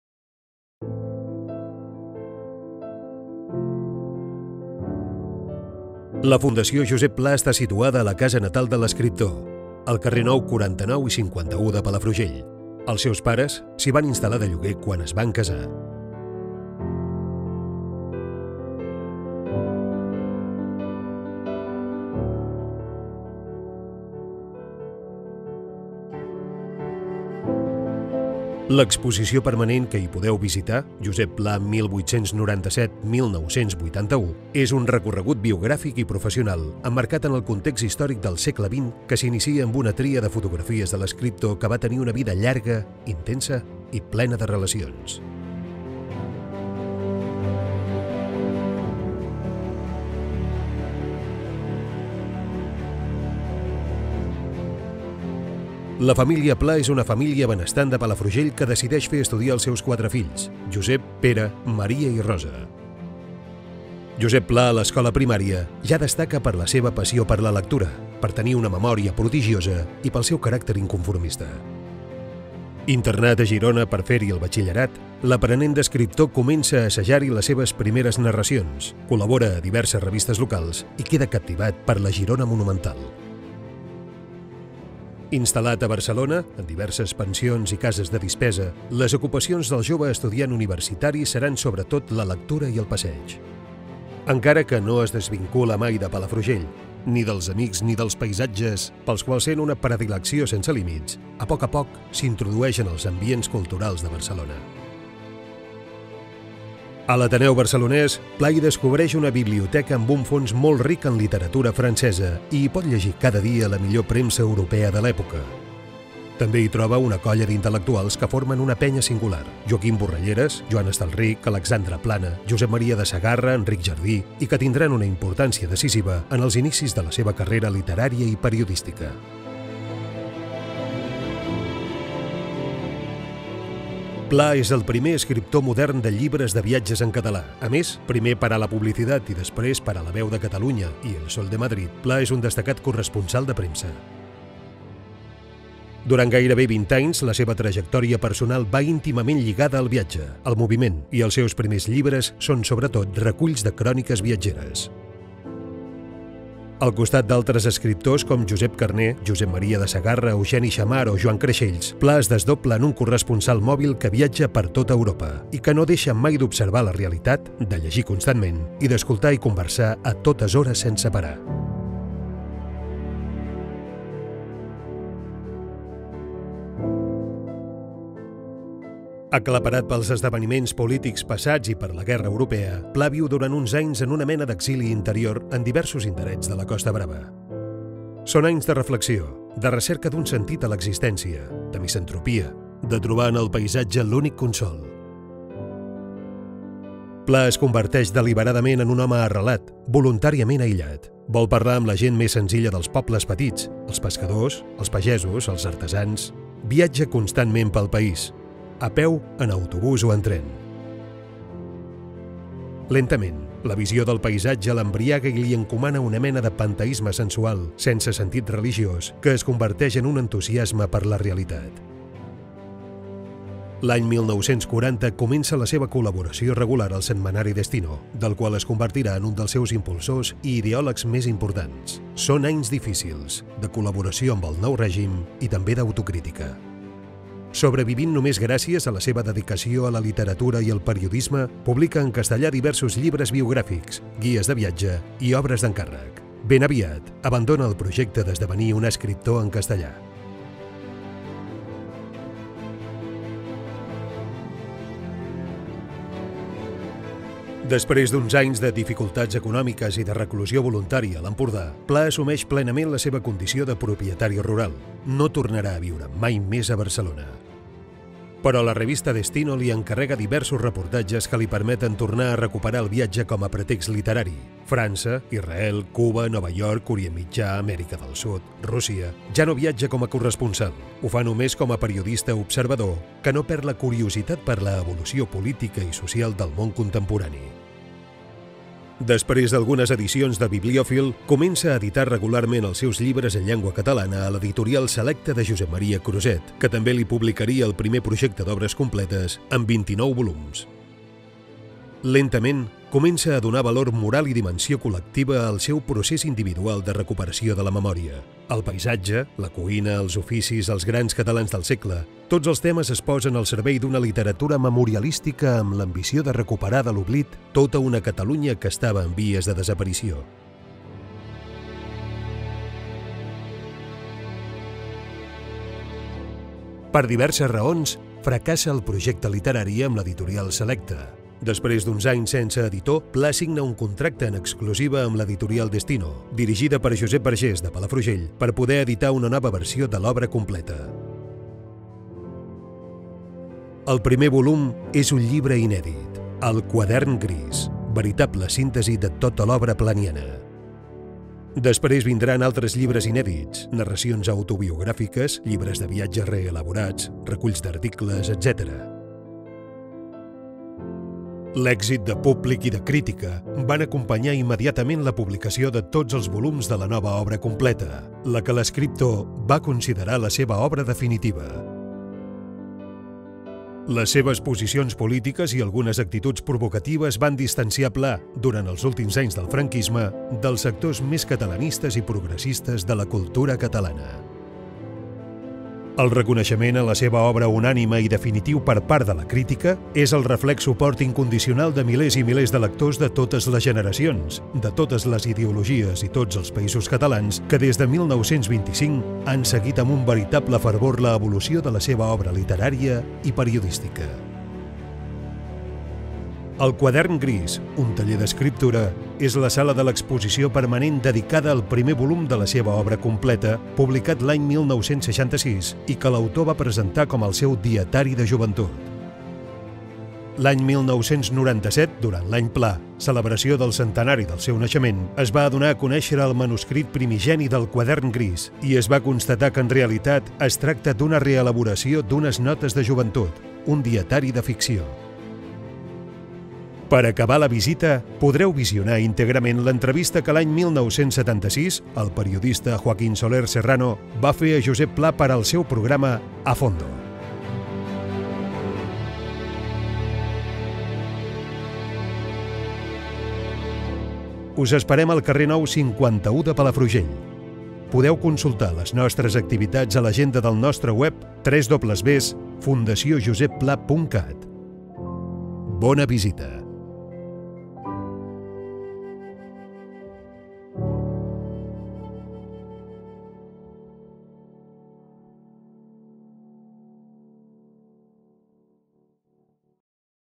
Visita virtual "La diabòlica il·lusió d'escriure"
La Visita virtual “La diabòlica il·lusió d’escriure” ressegueix el discurs  de l’exposició permanent de la Fundació “Josep Pla (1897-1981)” de manera detallada a través d’una veu en off. La càmera s’atura en els elements més importants de les vitrines que en documenten l’explicació.